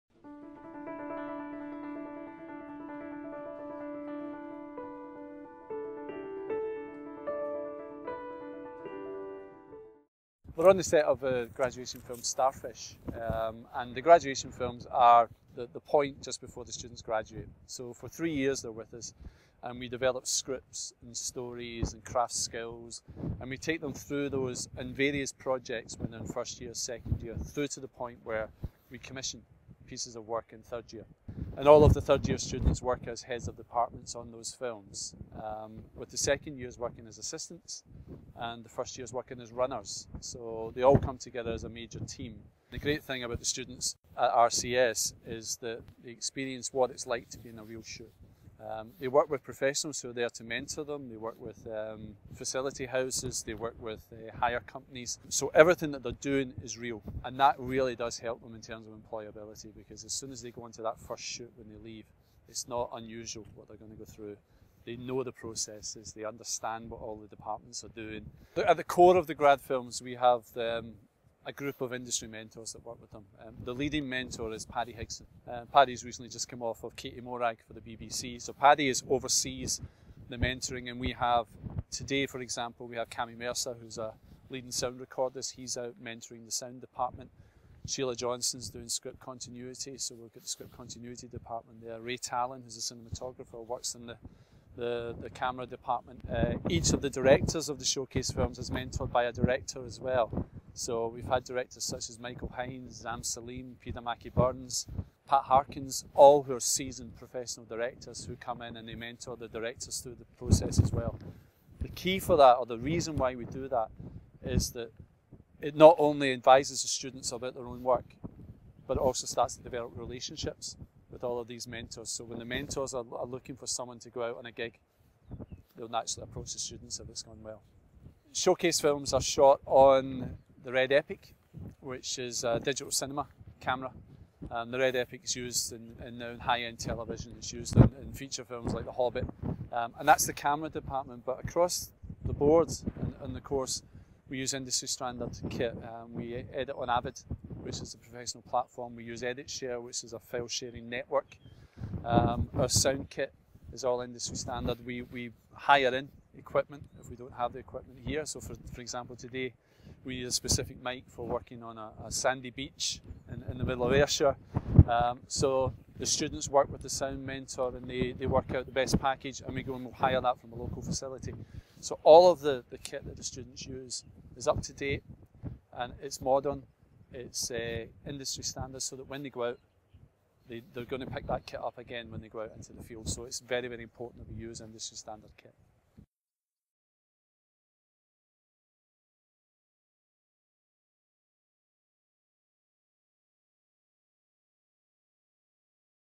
Focus on Tutors - Adam McIlwaine
Our Head of Screen, Adam McIlwaine talks about our Digital Film and Television degree programme whilst on location filming of one of our final year showcase films.